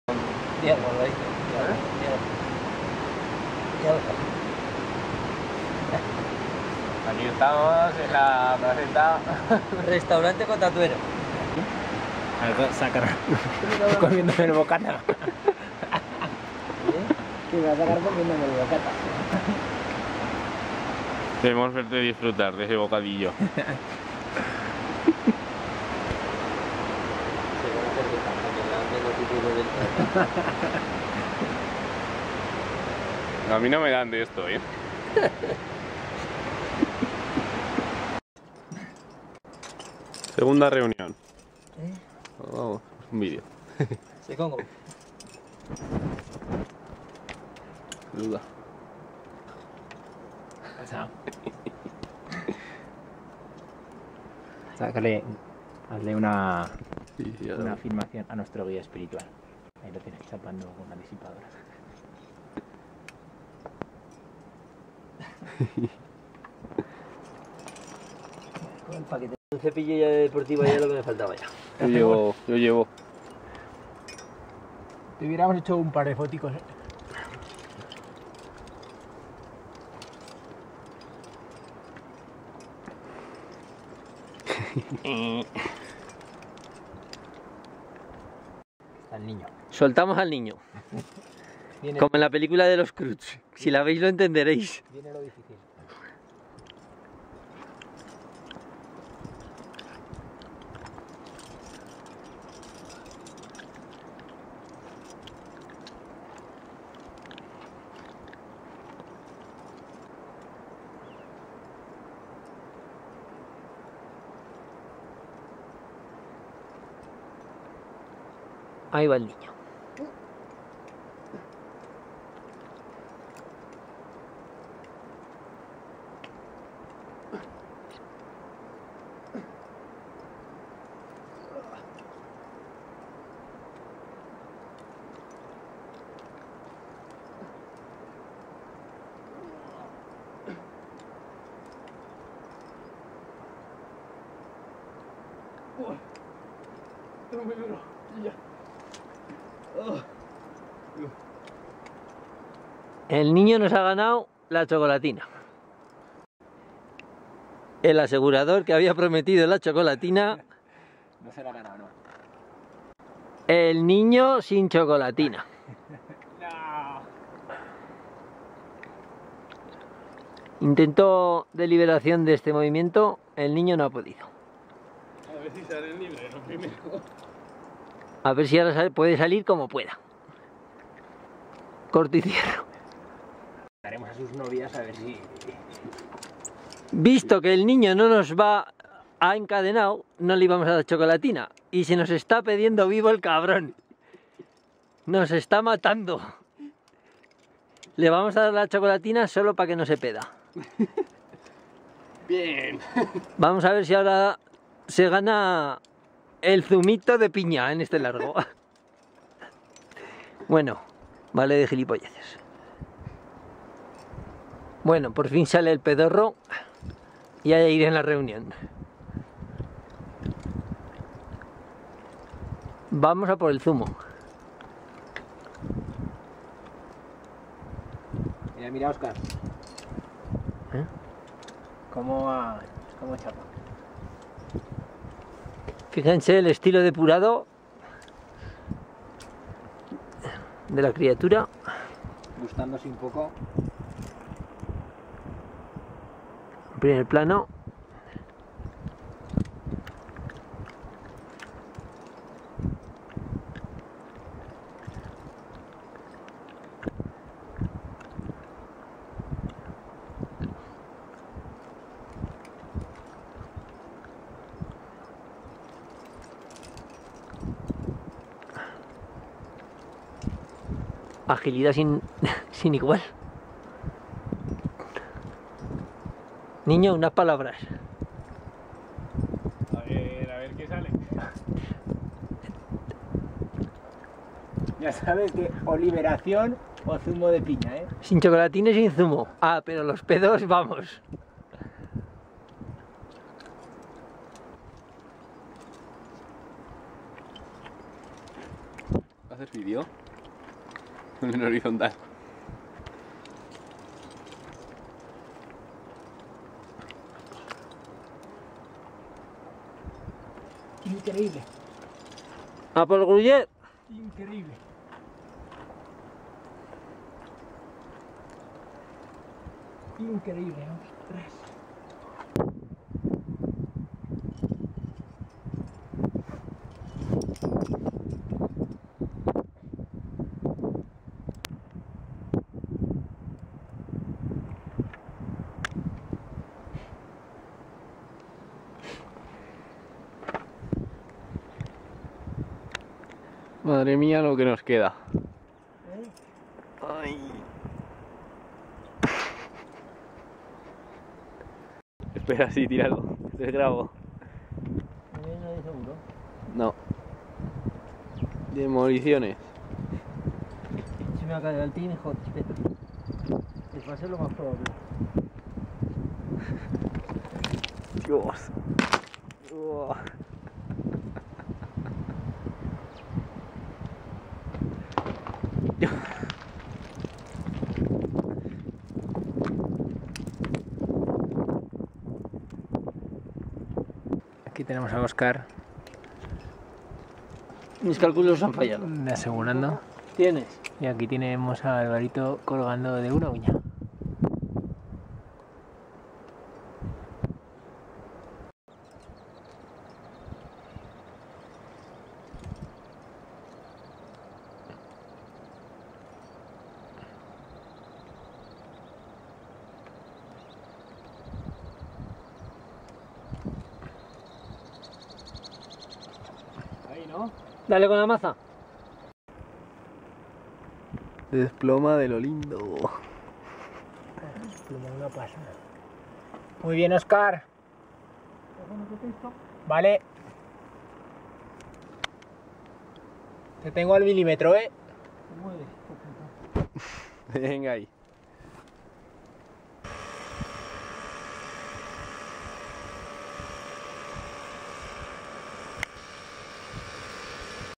Abriendo una vía en ordesa
Con humor a cargo de Oscar y Ramón ;)